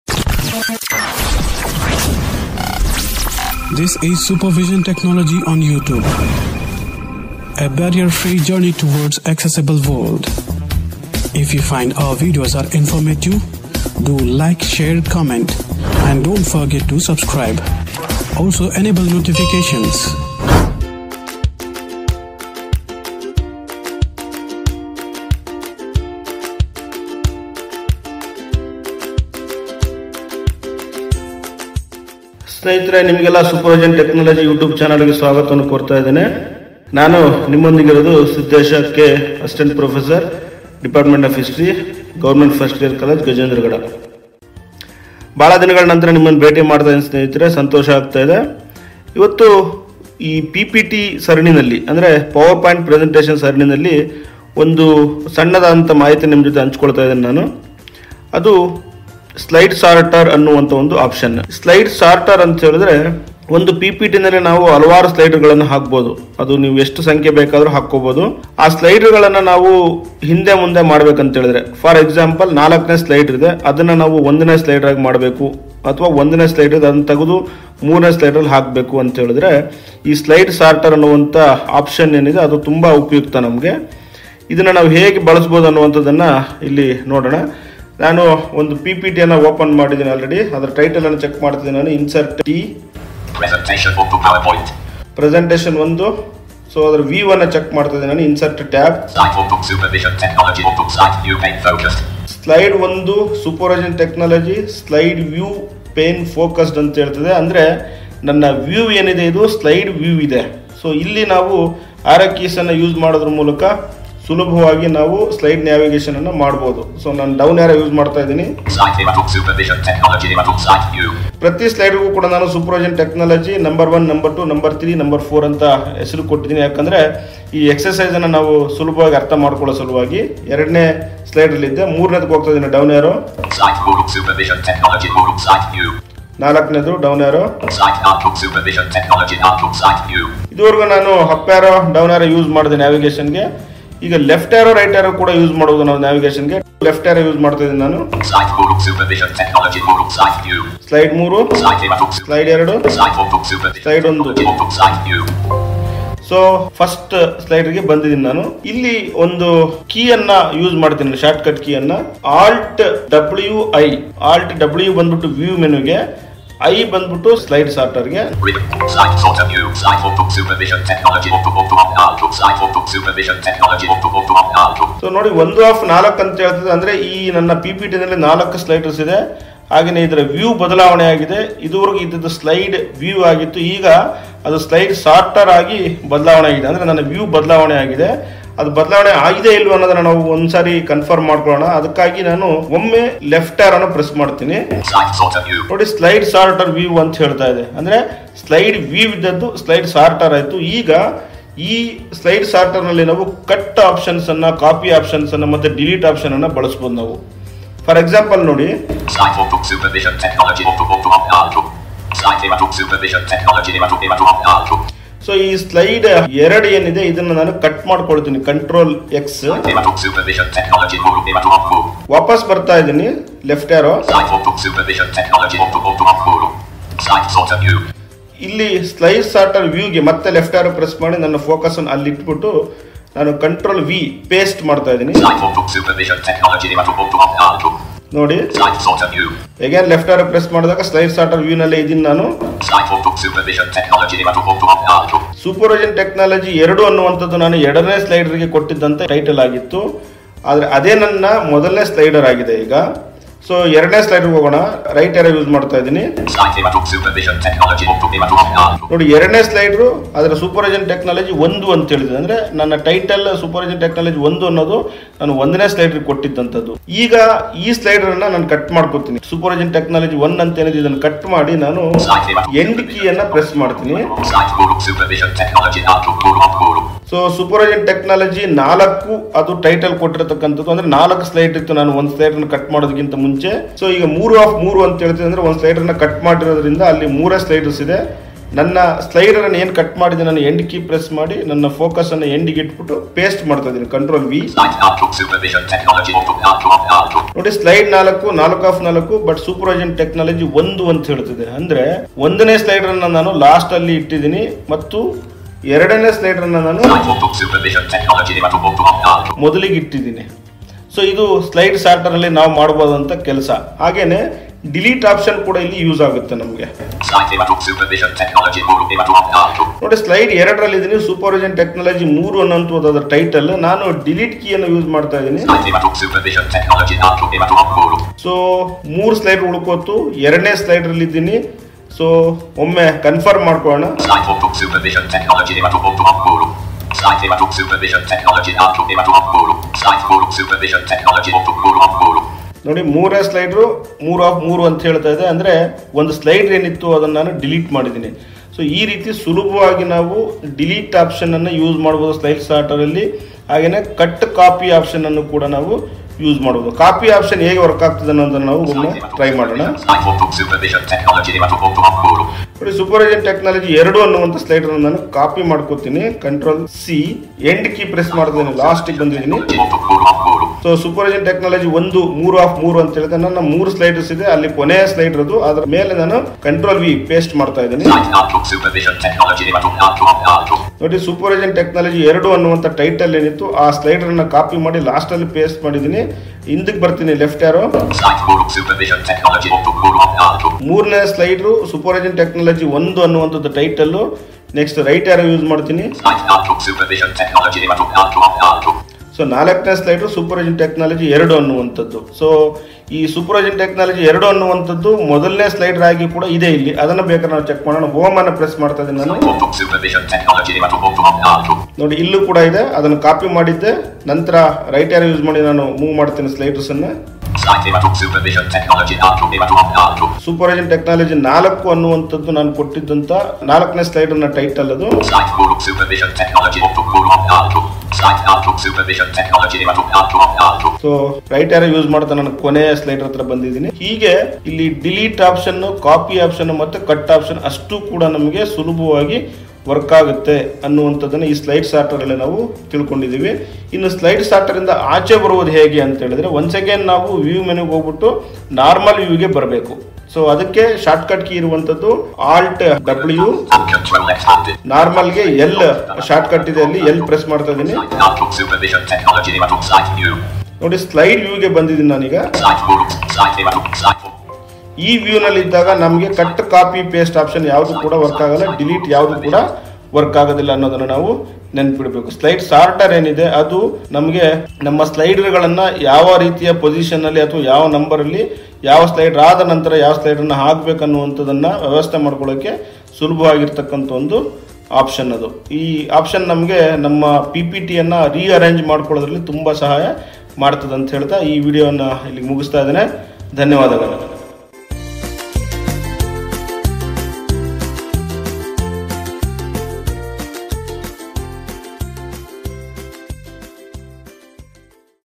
MS PPT episode 10, usage of Slide sorter | Siddesh K, using Screen Reader
Hi Friends,
Welcome to Super Vision Technology YouTube Channel.
in this tutorial, we are going to show you the very important trick for slides usage in PPT.
kindly watch.